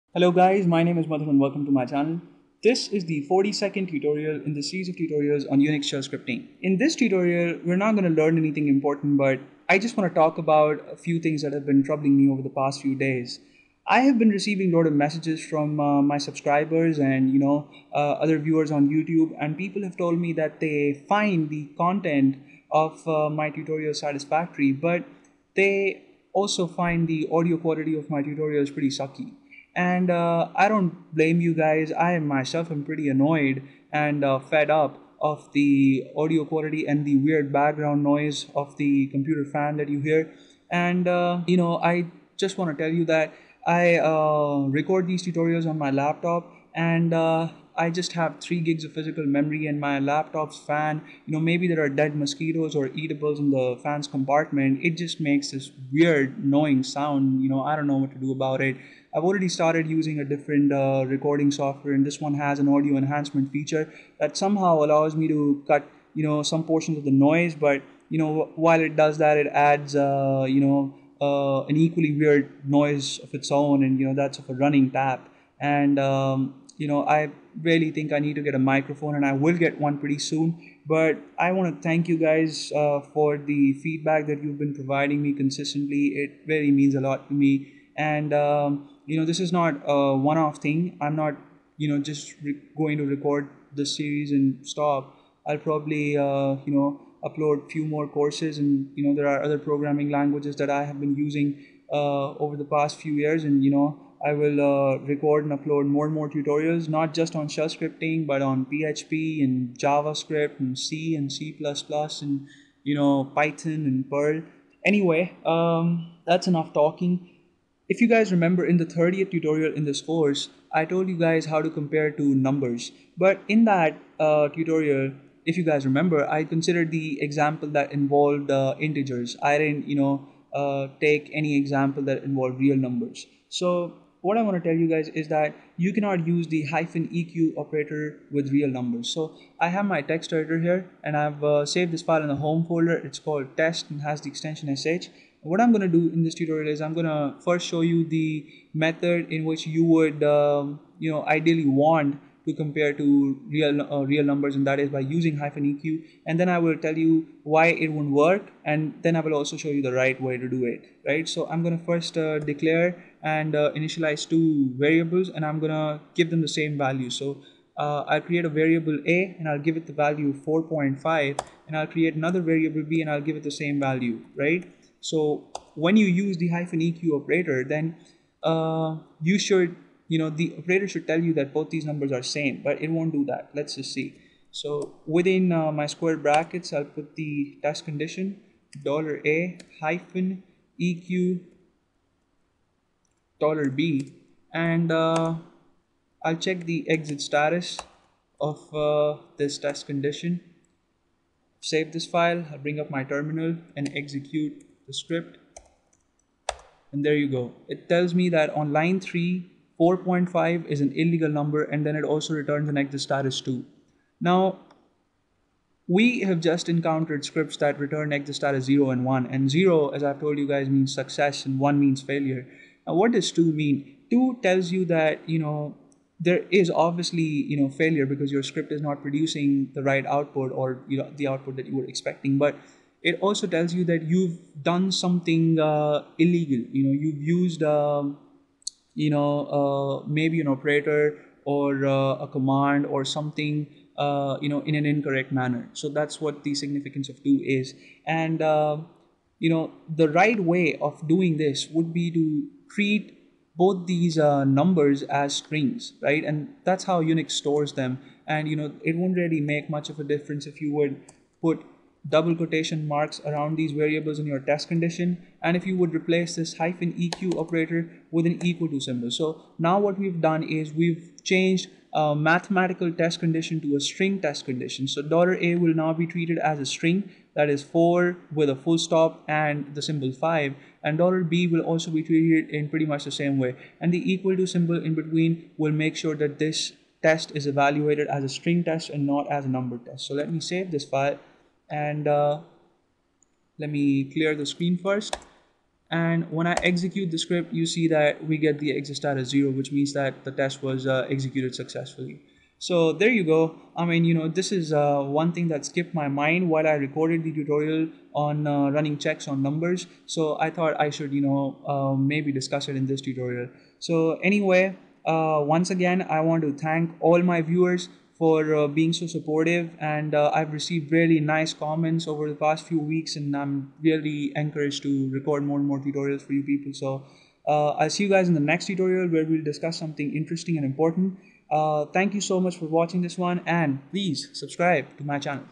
Shell Scripting Tutorial-42: Rant & Little Work
In this tutorial you'll learn to use the '=' operator while comparing two floats. The -eq operator does a good job with integers but it doesn't quite work with real numbers.